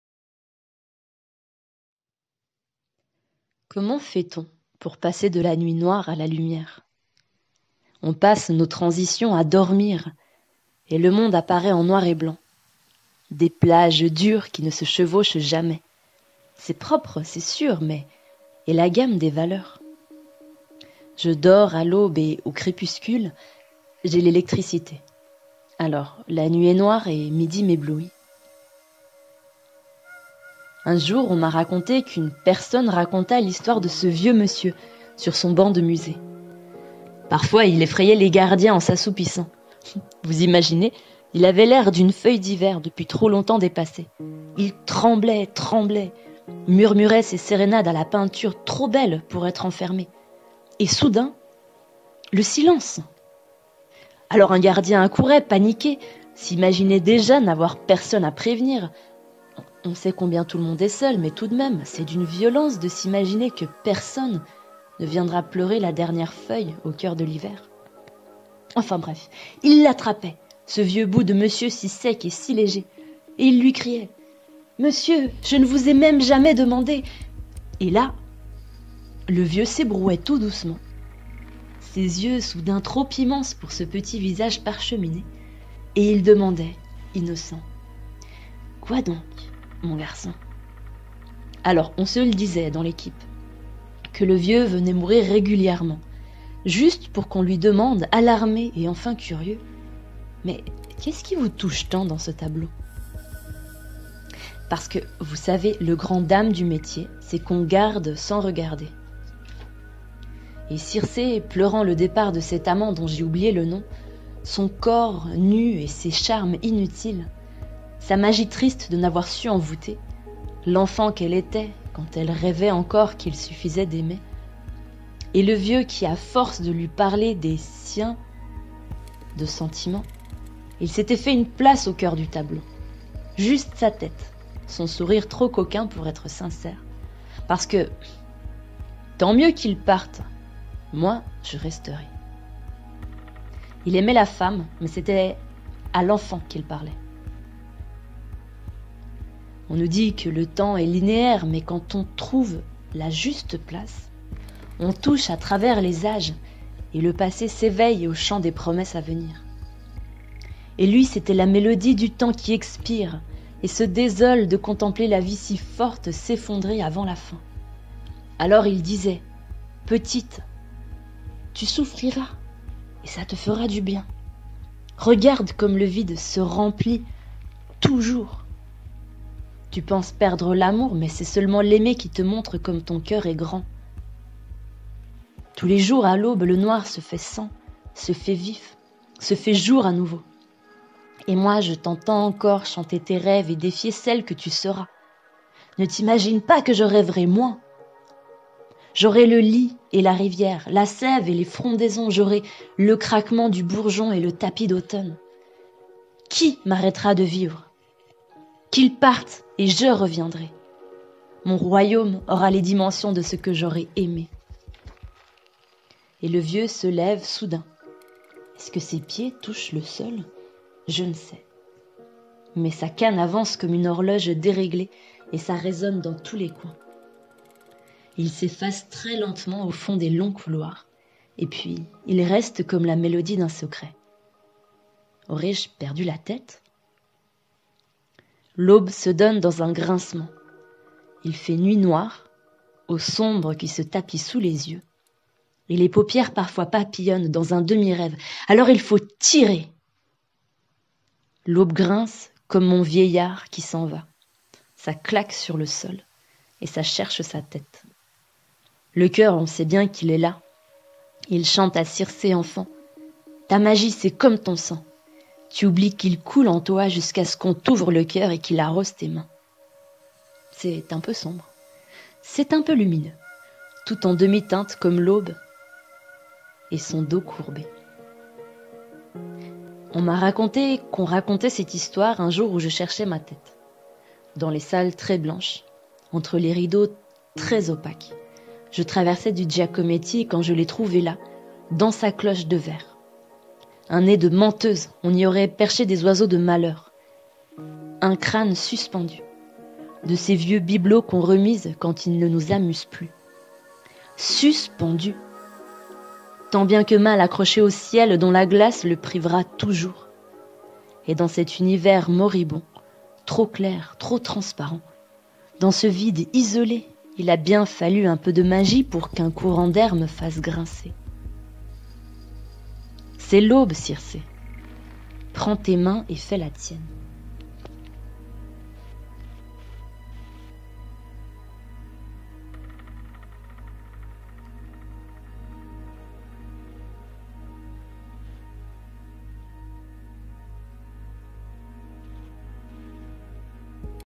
[Bribes] L'aube de Circé
Hello,

Un peu de poésie parce que j'avais envie :)

Des câlins

Vi'

PS : c'était la dernière page à colorier de mon carnet aquarelle 2022, donc très bientôt très longue vidéo pour un sketchbook tour :)